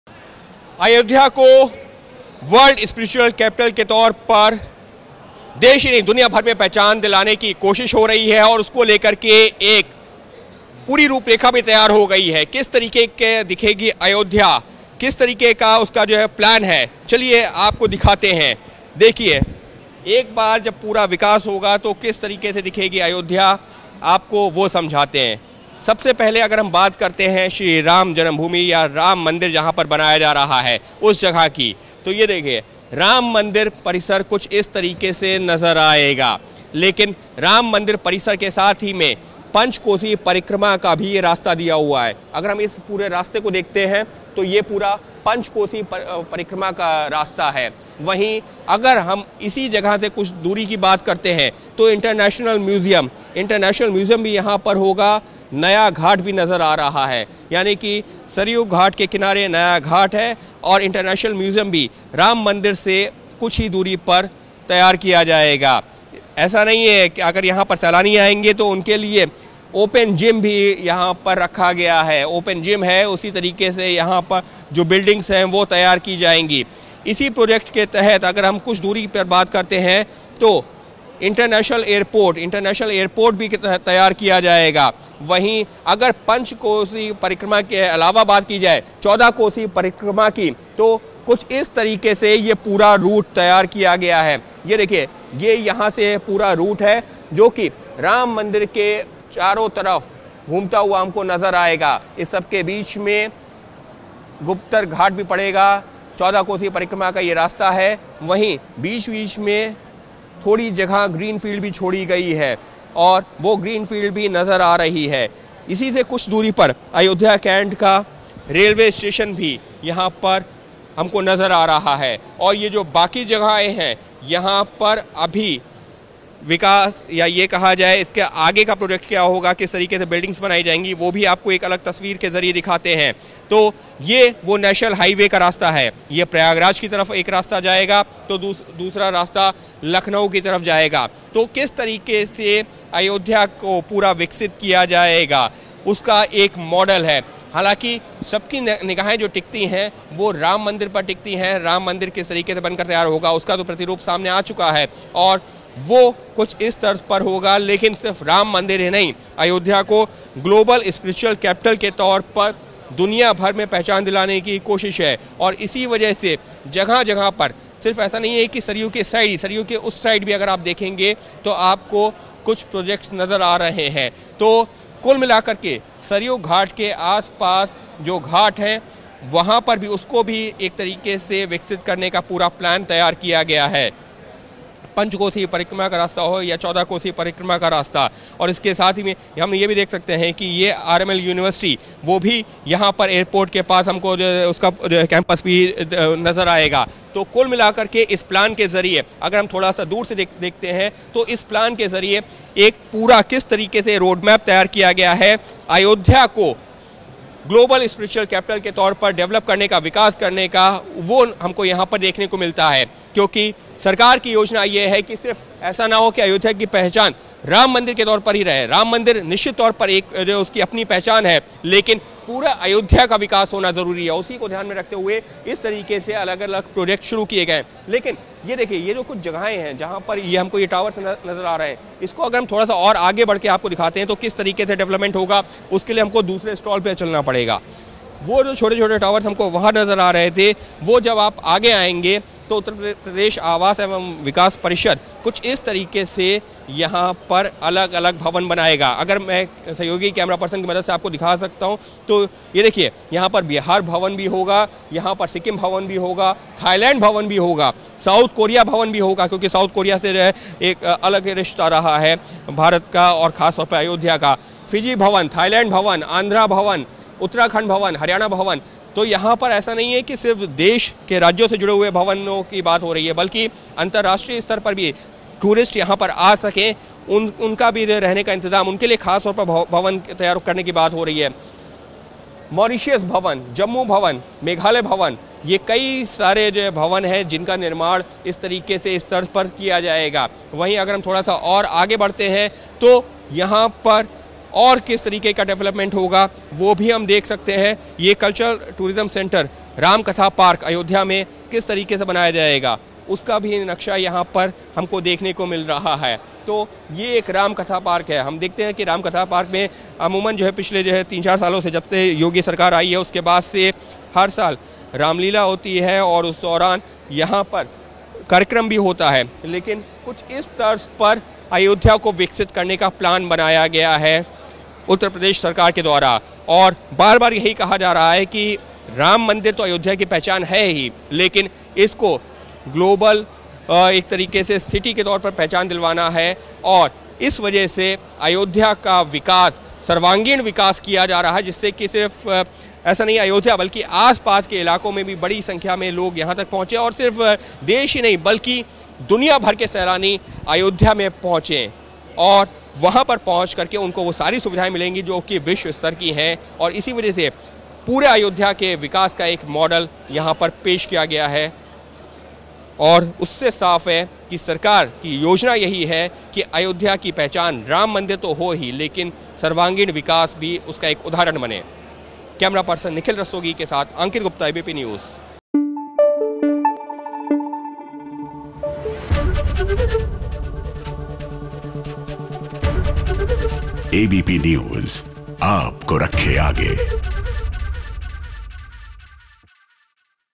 Ayodhya बनने जा रहा है विश्व धार्मक स्थल, तैयार किया गया पूरा मॉडल
अयोध्या की पहचान राम मंदिर तो है ही लेकिन कैसे अयोध्या को बनाया जाएगा विश्व धार्मिक राजस्थानी. इसी को दिखाते हुए एक मॉडल तैयार किया गया है जिसमें राम मंदिर के साथ ही पूरे अयोध्या को कैसे विकसित करना है वह दिखाया गया है.